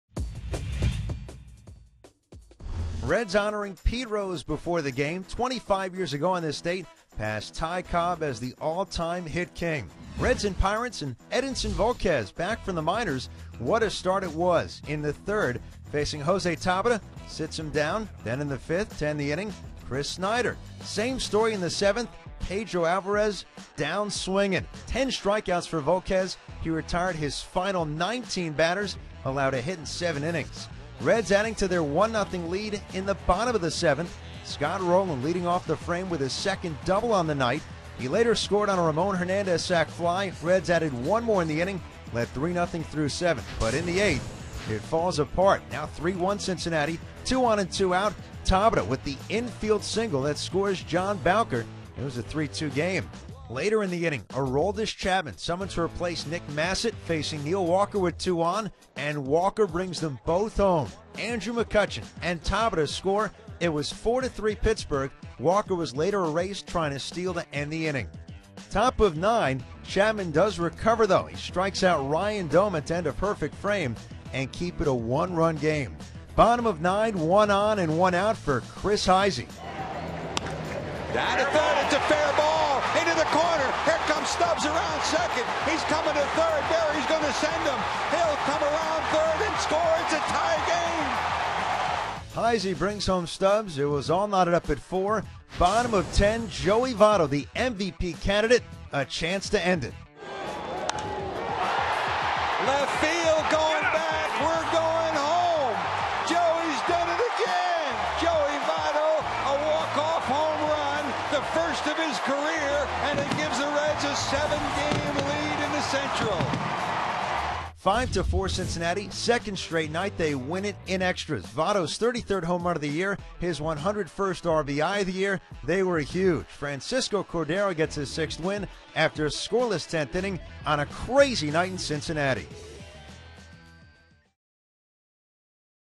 2010/09/11 Recap: PIT 4, CIN 5
9/11/10: Votto's first walk-off homer lifts Reds